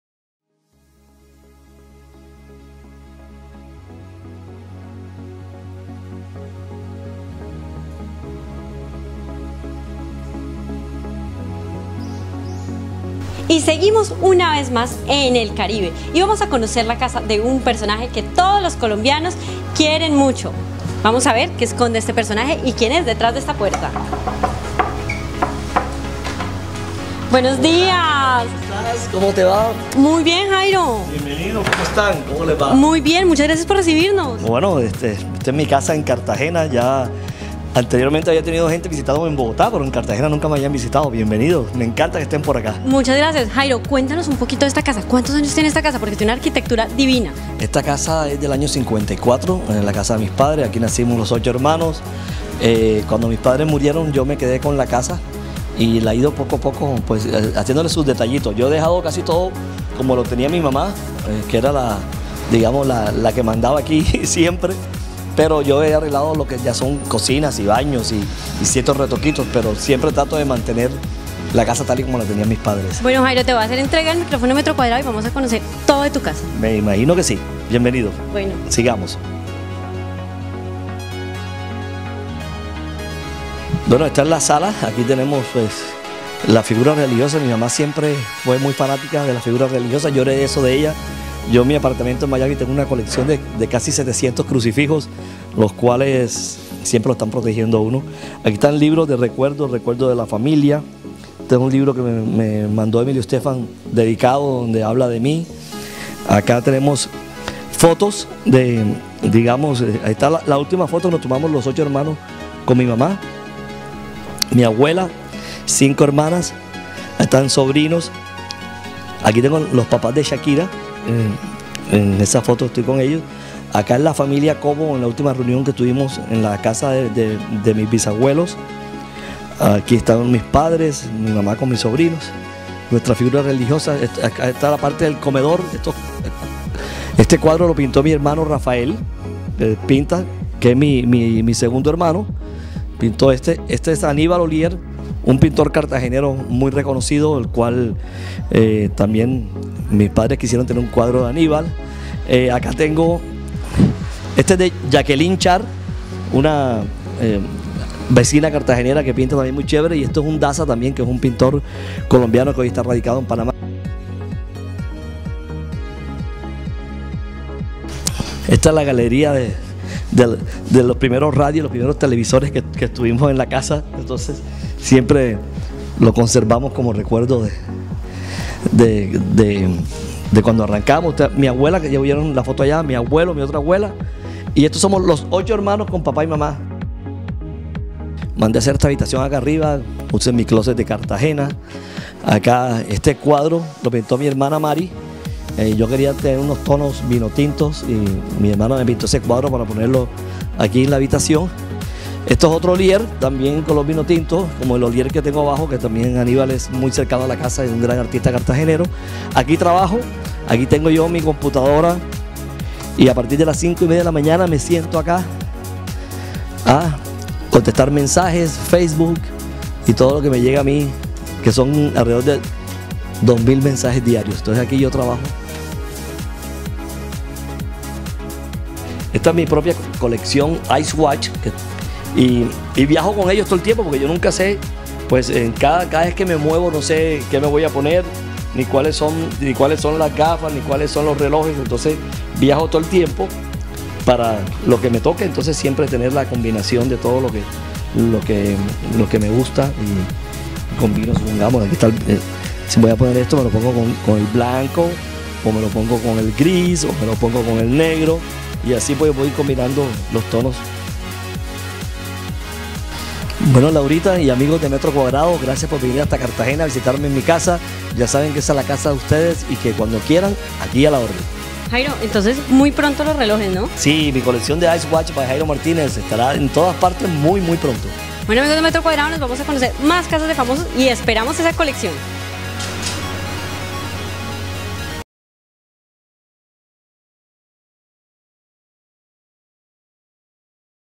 Jairo Martínez muestra su casa en Cartagena
Al entrar al hogar de Jairo lo primero que se percibe es la arquitectura tradicional del año 1954, allí en más de 500 metros cuadrados se conserva la imponencia de los arcos, techos en yeso, muros de hasta cuarenta centímetros y un patio central.

Estos son algunos apartes que usted verá en el vídeo:

- Arquitectura tradicional cartagenera.
- Remodelación de cocina y baños.
- En su sala nunca faltan las figuras religiosas.
- Fotos familiares y de amigos siempre presentes, al igual que    varios cuadros de artistas cartageneros.
- Libros con recuerdos de la familia Martínez.
- Galería de radios antiguos.
- Su habitación está decorada con un cuadro pintado por su  hermana.